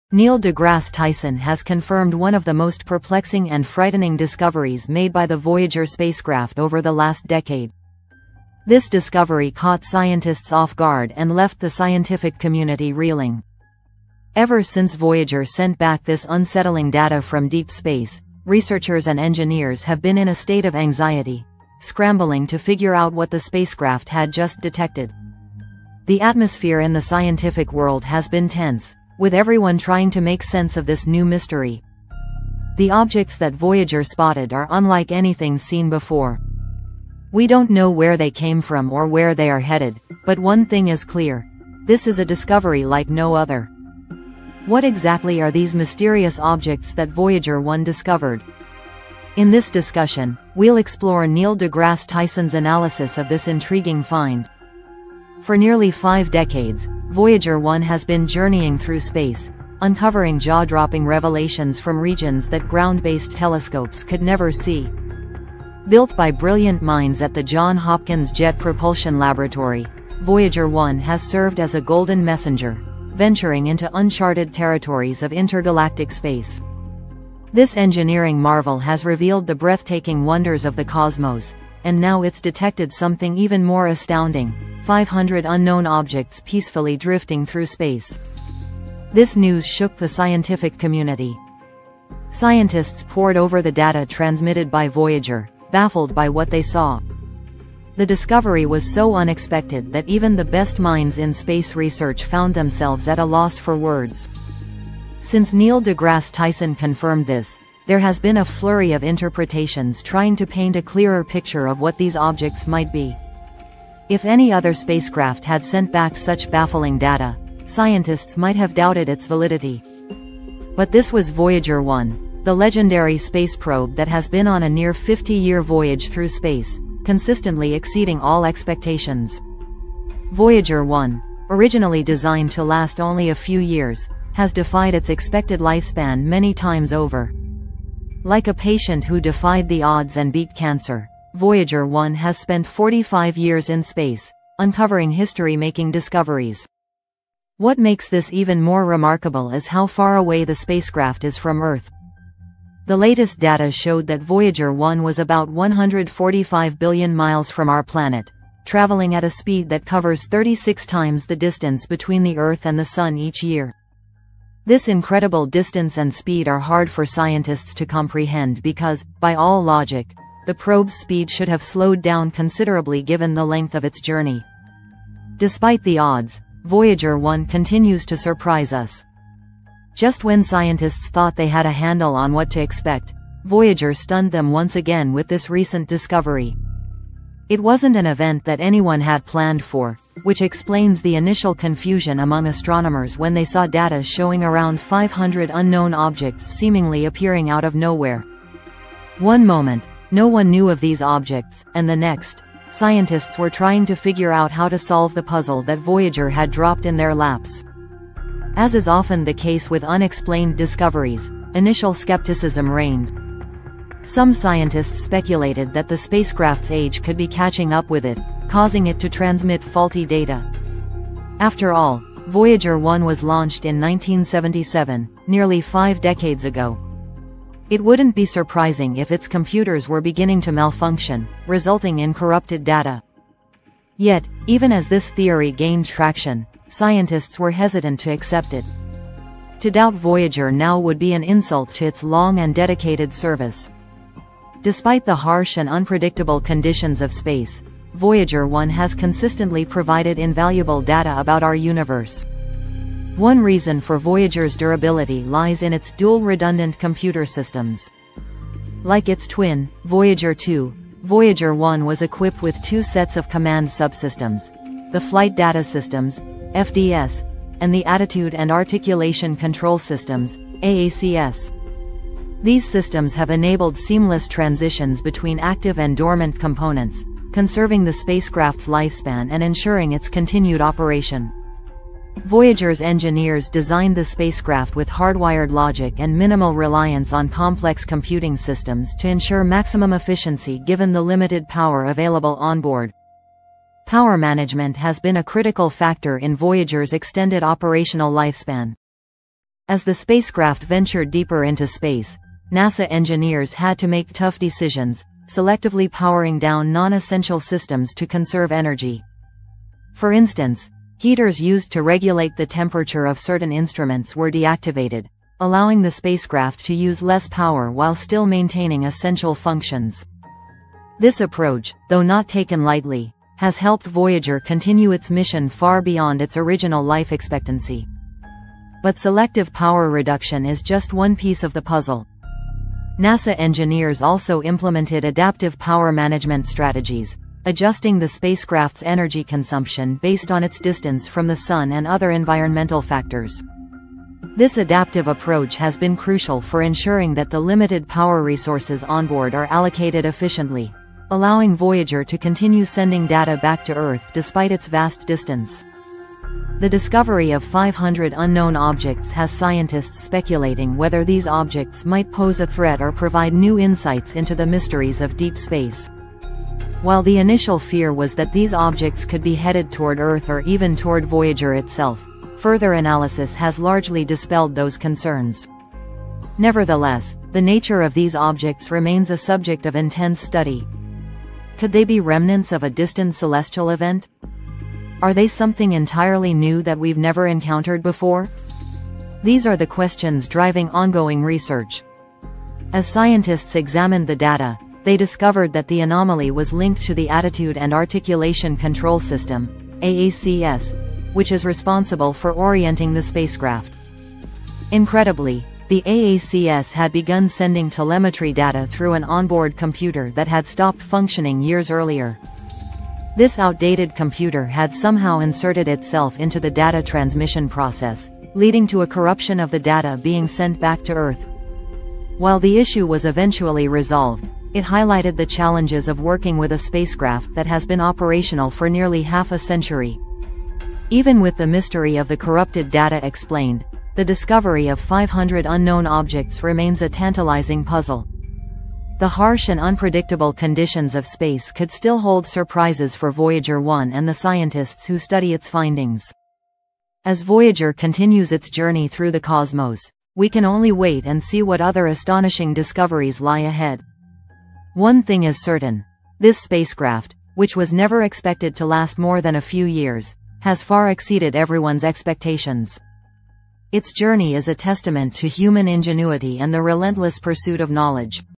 "Neil deGrasse Tyson Breaks News: Voyager 1 Detects 500 Unknown Objects in Space!"
Neil deGrasse Tyson breaks down the incredible discoveries made by Voyager 1, which has detected 500 unknown objects passing by in space. What do these strange objects mean for our understanding of the universe? Could they be signs of extraterrestrial life or completely new cosmic phenomena? Join us as Neil deGrasse Tyson explores the details behind Voyager 1’s latest findings, providing expert analysis on these puzzling discoveries and what they could reveal about the mysteries of deep space. Watch now to dive into the latest breakthroughs in space exploration and scientific discovery!